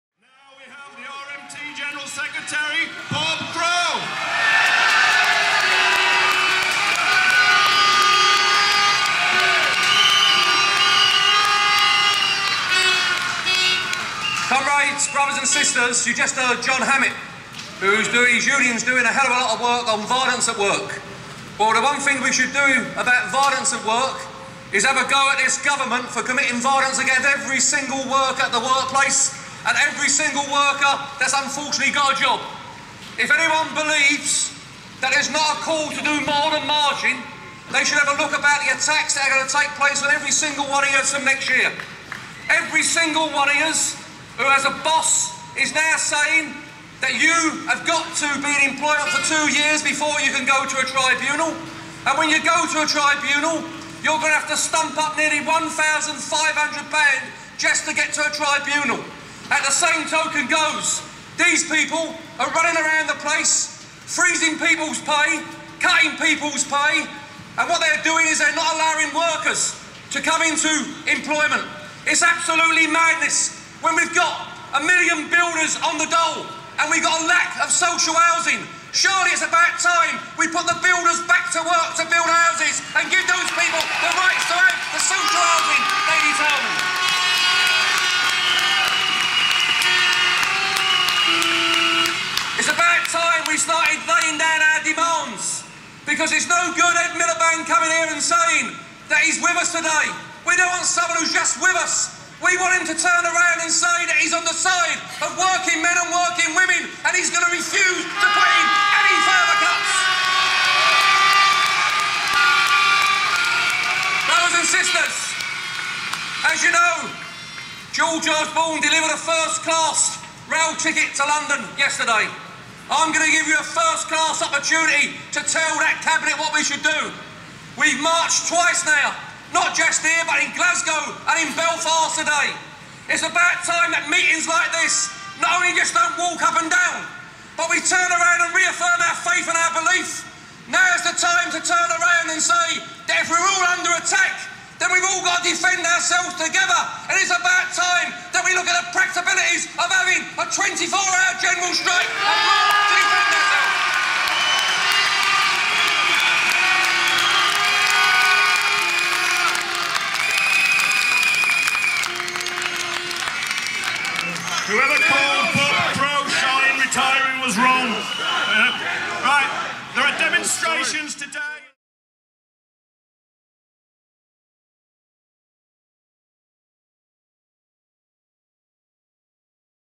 Bob Crow TUC Demo - Socialist Party
The call for preparing for a 24 hour General Strike, made by Bob Crow RMT General Secretary at the TUC demonstration and rally in Hyde Park, London on 20 October 2012.